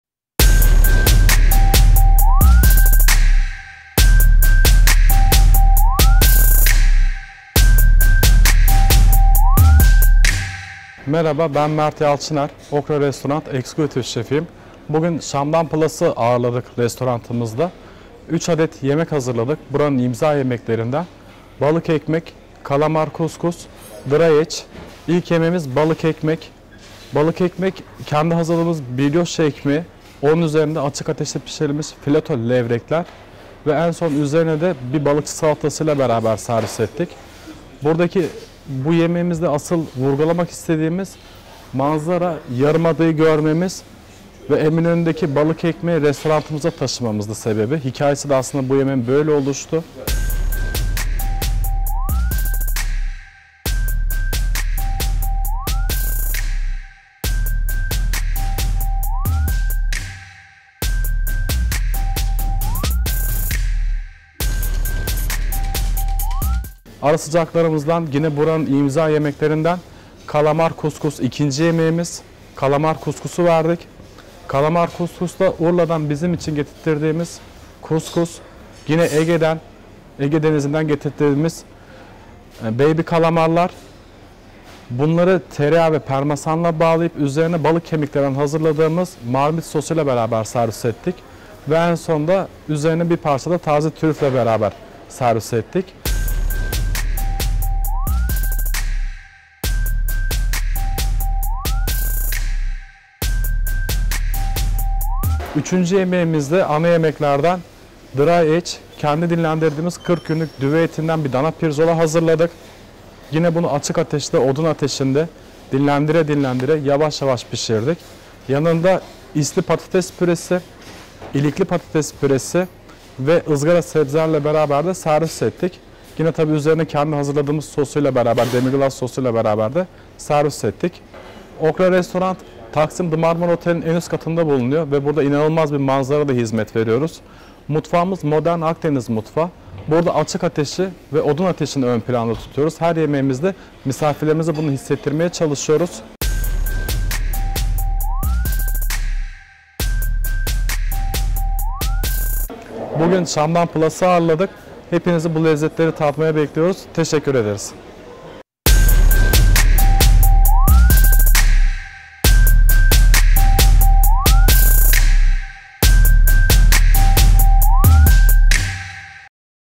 Mert Yalçıner
Şef Mert Yalçıner, genç yaşına rağmen mutfakta başarılı bir kariyere sahip. İstanbul'un en özel manzaralarından birine sahip  restoranın şefliğini yapan Mert Yalçıner ile mutfakta bir araya geldik. Mert şef imza lezzetlerinin sırlarını Şamdan Plus okuyucuları ile paylaştı.

Röportaj: Melih Türk
Video: Cafer Pala